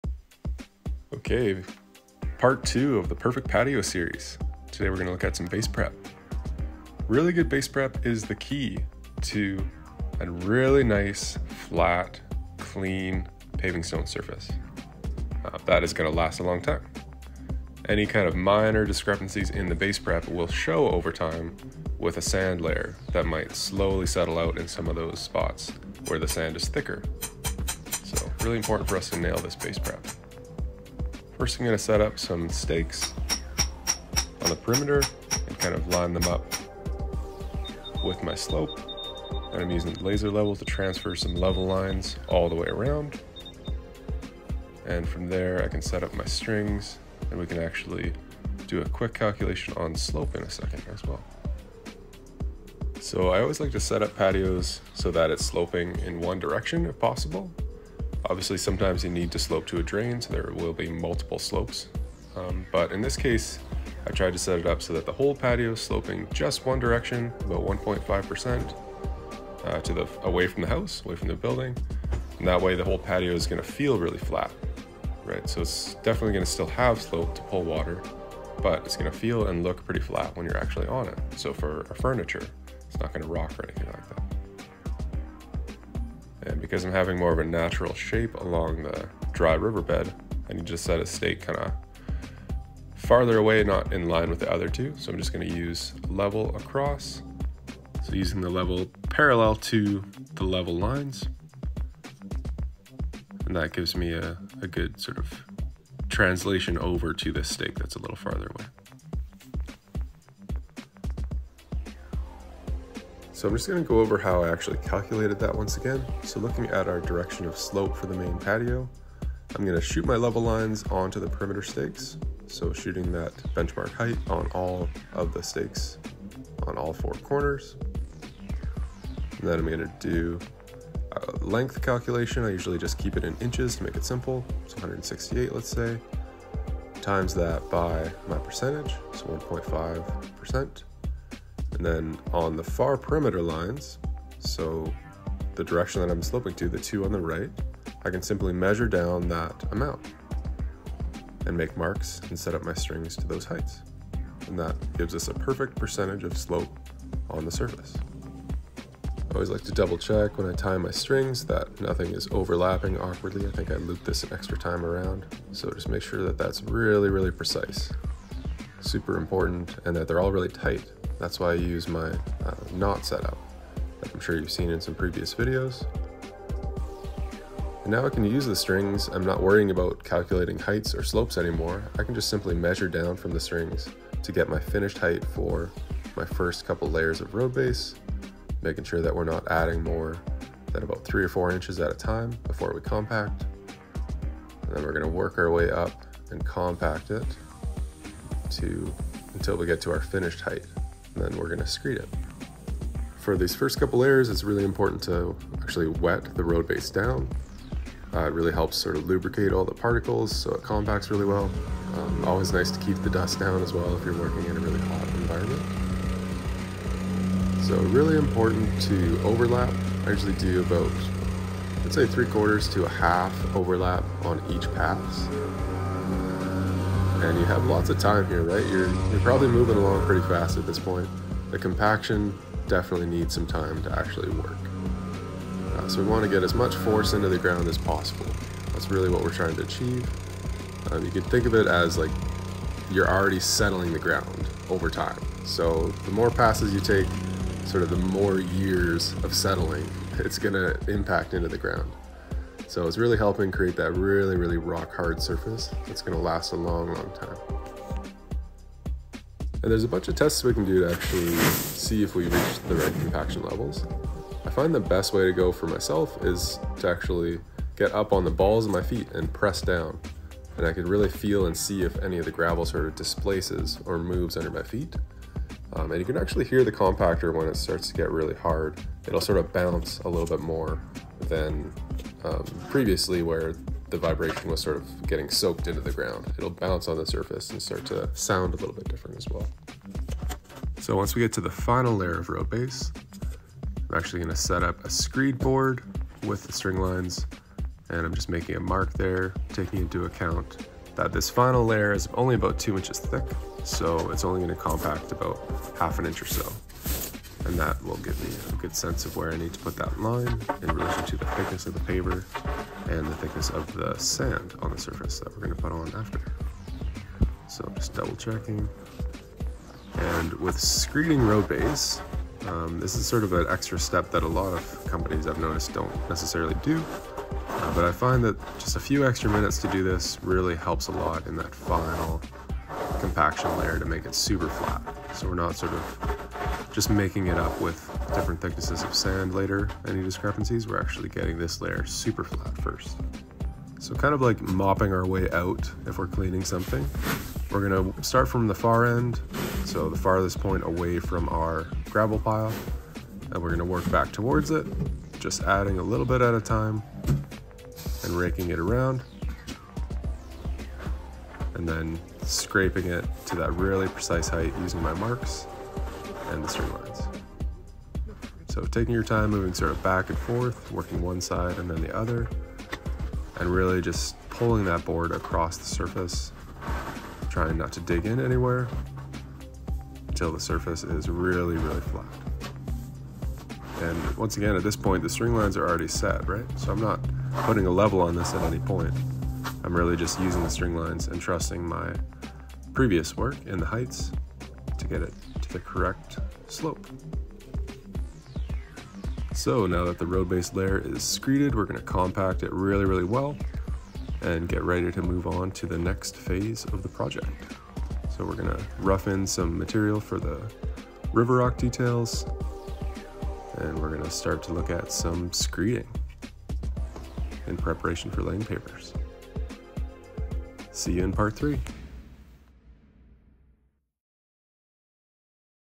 How to level the ground for BASE PREP for Pavers! The Perfect Patio series Part 2: Base Prep
To create the PERFECT PAVING STONE patio, we must take our time learning how to level the ground with the base prep. Base prep is essential to our patios longevity and for creating a superbly flat surface that drains well. Let’s check out how to do it the right way!

Let me know what you think and let talk about it!